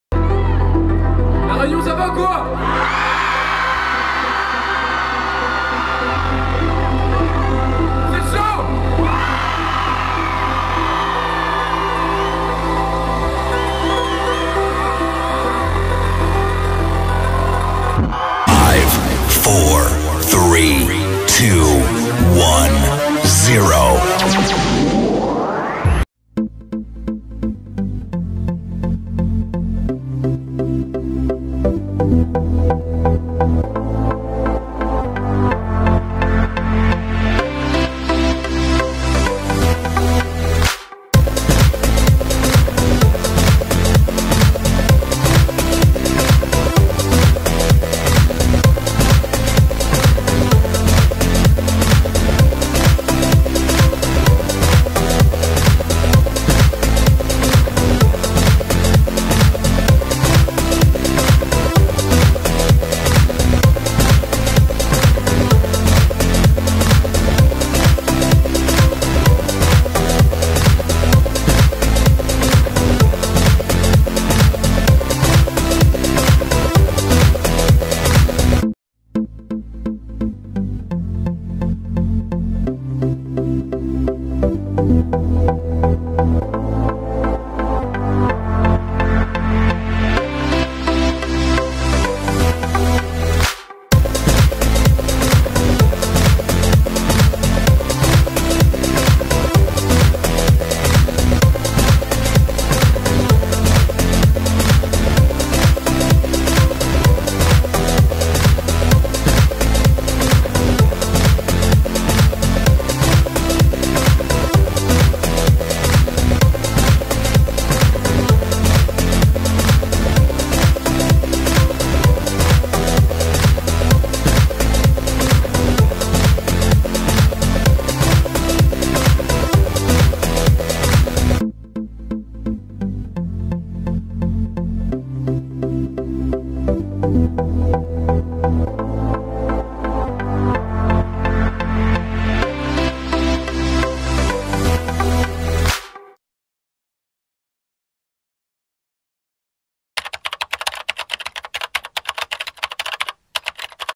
Souvenirs du Sakifo Muzik Festival 2018
L’instant est béni. Tout le reste est souvenir. Voici quelques souvenirs en images de la dernière édition du Sakifo Muzik Festival 2018 qui s’est déroulé du 1er au 3 juin à Saint-Pierre de La Réunion.